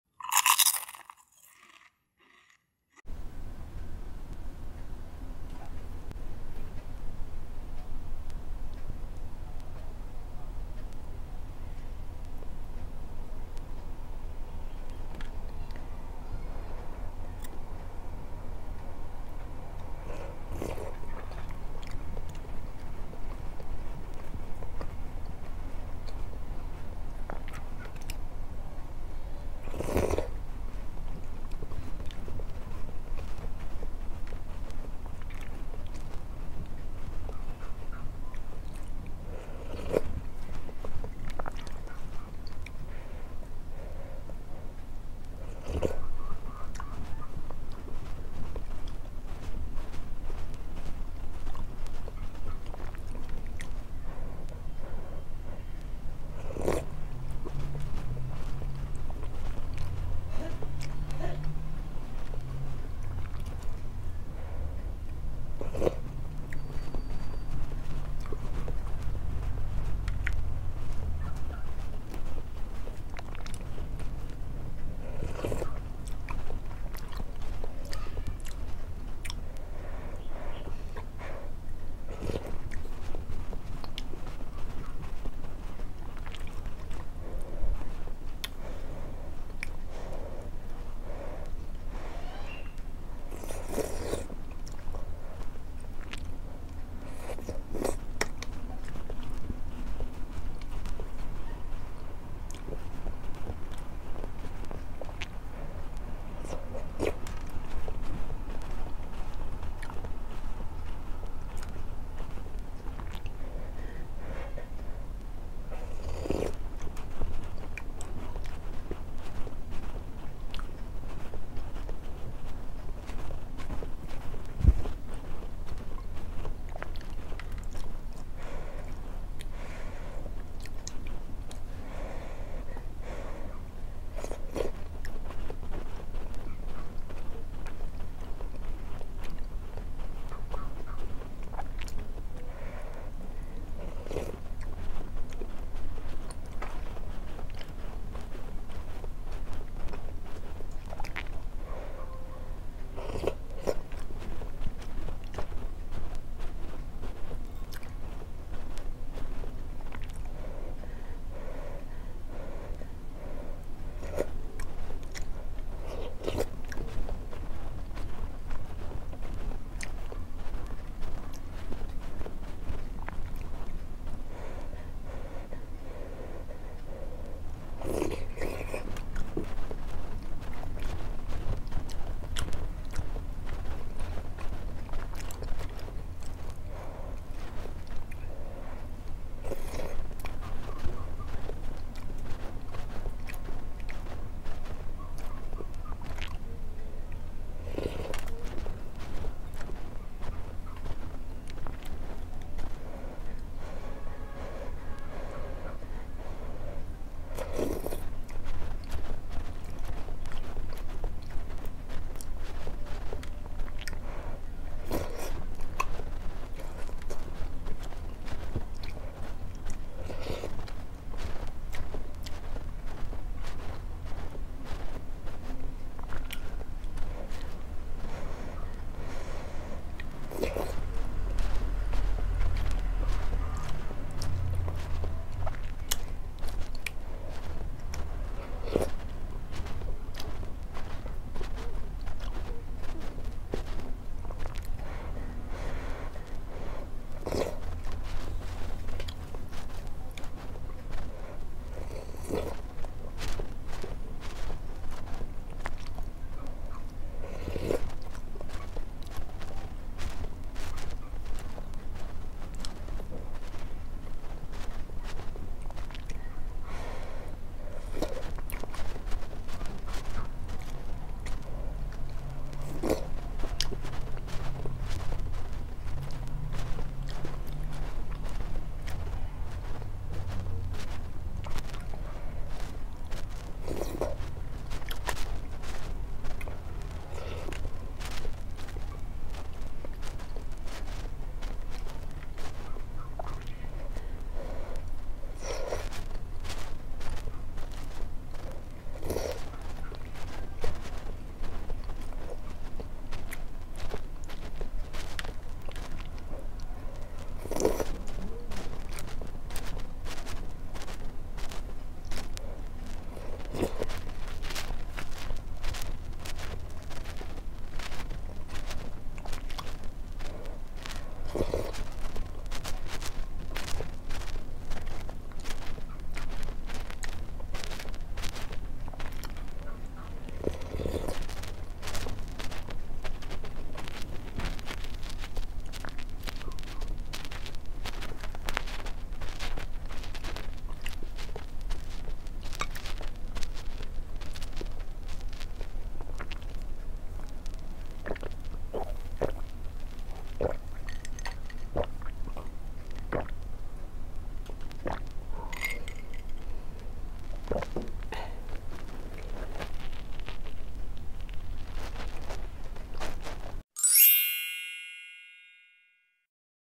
ASMR|SINDUL/SIYURUH|TAUSUG FOOD|SWEET STICKY SOUP|MUKBANG|NO TALKING
Hello today is my ASMR mukbang of eating Sindul a sweet sticky soup. Hope you like my mukbang. Thank you for your supports.

NOTE: This is an ASMR video. To experience ASMR tingles, PLEASE WEAR HEADPHONES. If you do not enjoy EATING SOUNDS, DO NOT WATCH.